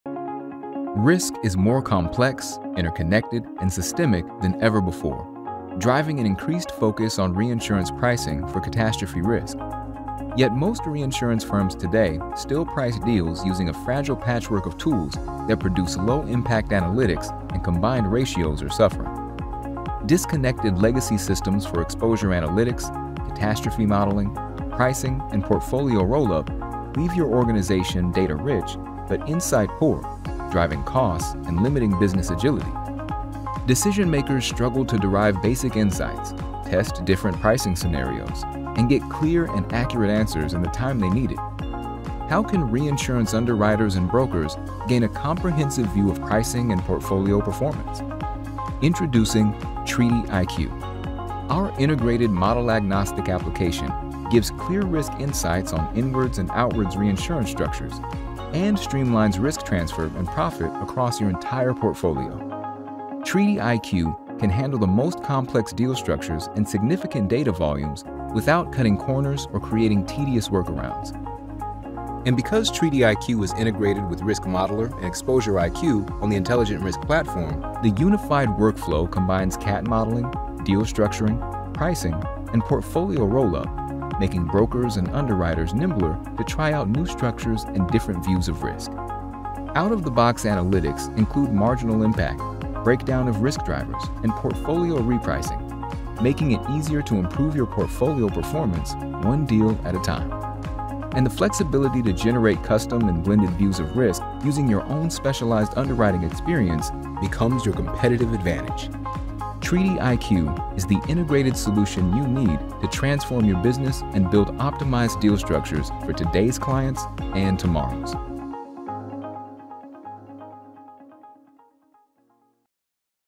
Moody’s TreatyIQ
As risk grows more complex, companies benefit from a unified approach to reinsurance pricing. TreatyIQ, integrated with Risk Modeler and ExposureIQ, offers clear insights, streamlines workflows, and helps brokers and underwriters optimize portfolio performance for a competitive advantage.